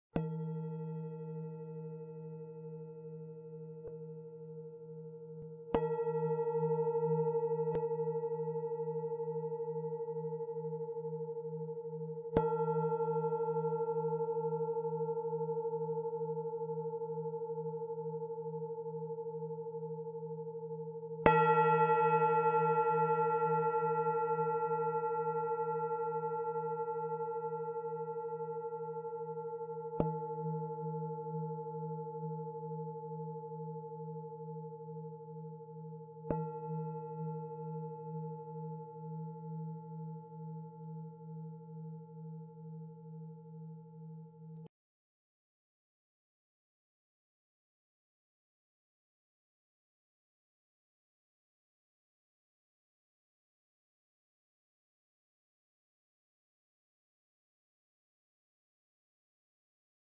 1 BOL CHANTANT TIBETAIN 2371
Son du bol chantant 2371 , à découvrir sur notre site.Bonjour, dans cette vidéo je vais vous présenter le bol chantant bouddhiste tibétain, un objet musical très intéressant. Vous pouvez le placer sur votre bureau, et vous avez la possibilité d'avoir différents effets sur vous-même, en fonction de la fréquence. Encore une fois, comme d'habitude, merci de liker et de partager.